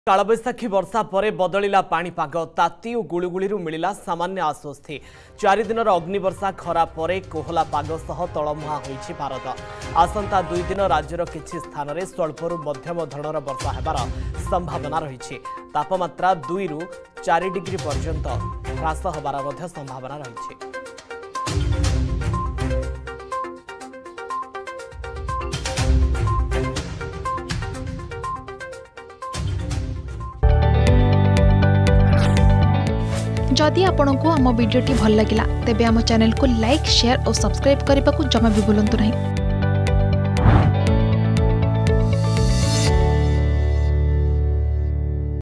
କାଳବୈଶାଖୀ ବର୍ଷା ପରେ ବଦଳିଲା ପାଣିପାଗ || Weathers Update
କାଳବୈଶାଖୀ ବର୍ଷା ପରେ ବଦଳିଲା ପାଣିପାଗ । ତାତି ଓ ଗୁଳୁଗୁଳିରୁ ମିଳିଲା ସାମାନ୍ୟ ଆଶ୍ବସ୍ତି ।